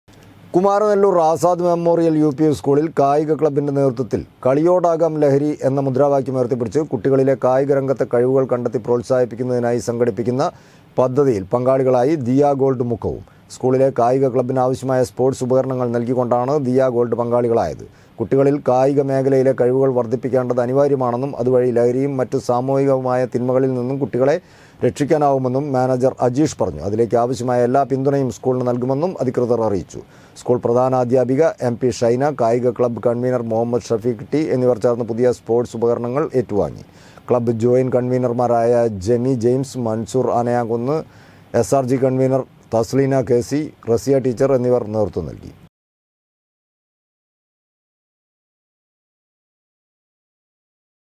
കുമാരനല്ലൂർ ആസാദ് മെമ്മോറിയൽ യുപി സ്കൂളിൽ കായിക ക്ലബ്ബിൻറെ നേതൃത്വത്തിൽ കളിയോടാകാം ലഹരി
കുമാരനല്ലൂർ ആസാദ് മെമ്മോറിയൽ യുപി സ്കൂളിൽ കായിക ക്ലബ്ബിൻറെ നേതൃത്വത്തിൽ  കളിയോടാകാം ലഹരി എന്ന  മുദ്രാവാക്യമുയർത്തിപ്പിടിച്ച് കുട്ടികളിലെ കായിക രംഗത്തെ കഴിവുകൾ കണ്ടെത്തി പ്രോത്സാഹിപ്പിക്കുന്നതിനായി സംഘടിപ്പിക്കുന്ന പദ്ധതിയിൽ പങ്കാളികളായി